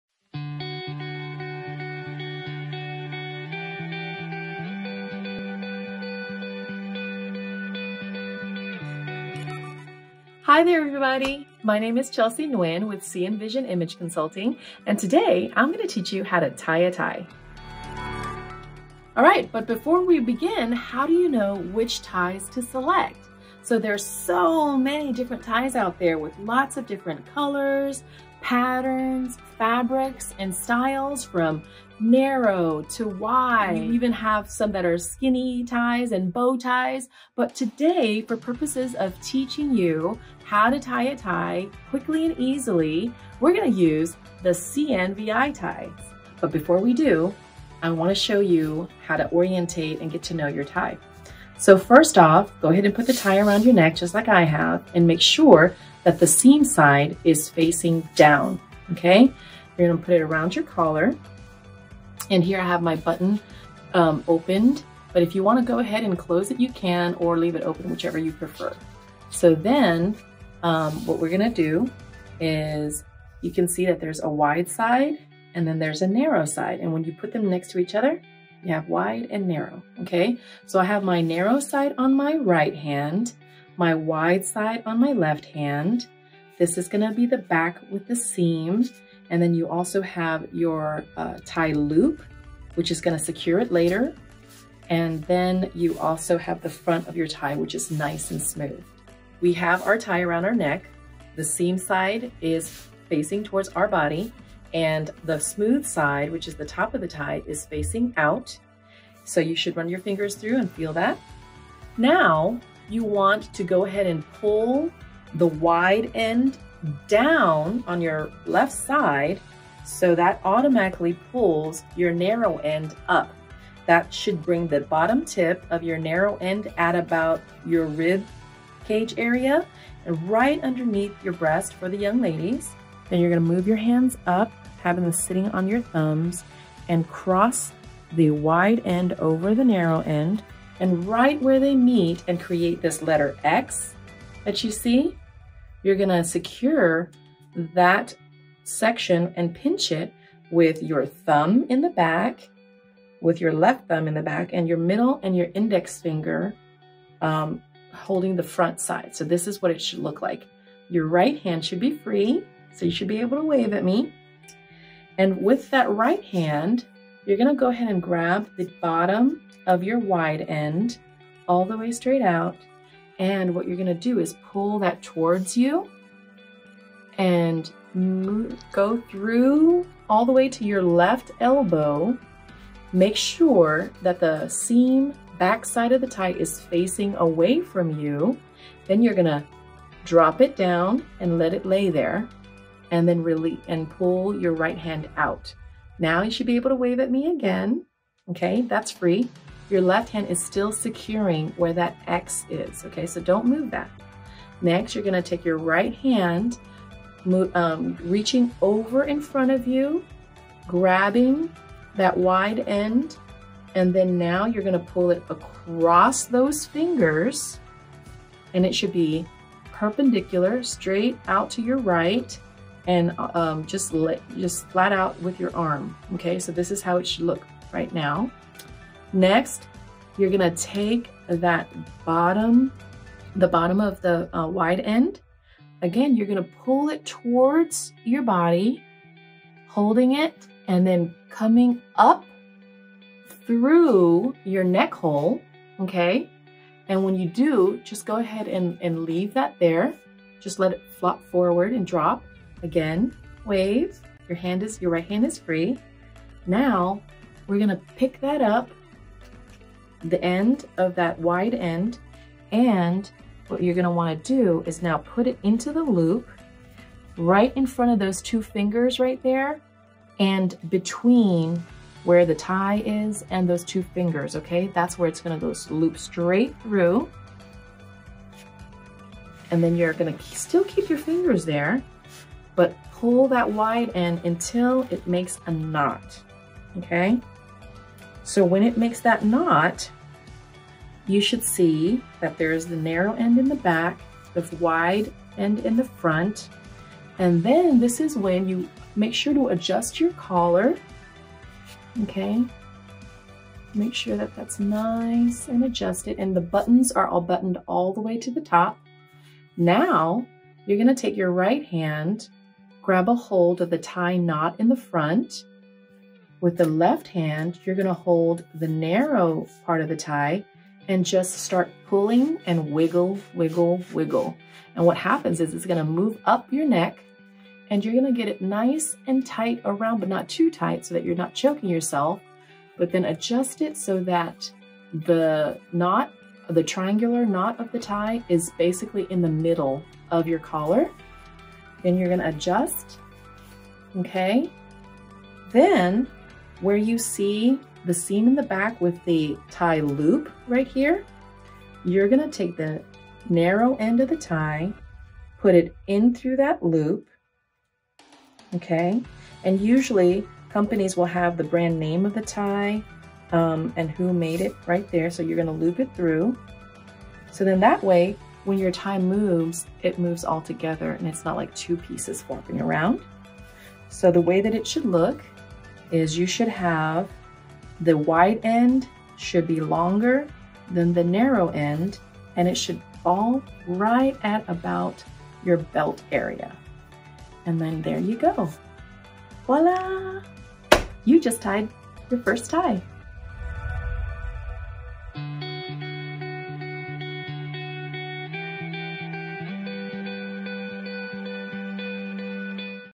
Tying The Tie: A Step-By-Step Guide by Chelsea Nguyen
Join CNVI Director & Founder, Chelsea Nguyen and learn how to create a beautiful, sleek and professional ponytail in 3 steps.

This video is part of a full custom CNVI lesson plan for adaptive hairstyle techniques for all abilities.